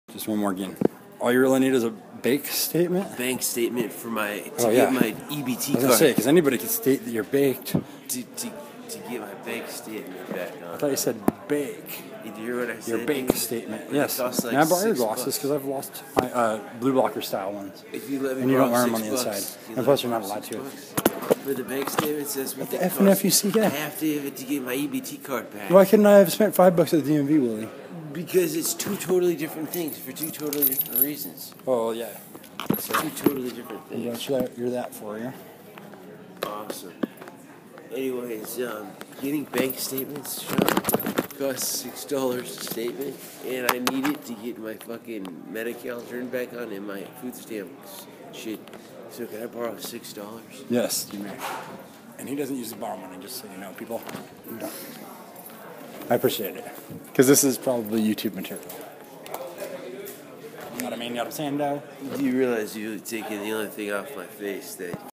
My friend Willi
Funny freestyle stuff at the evil Empire establishment known as chase bank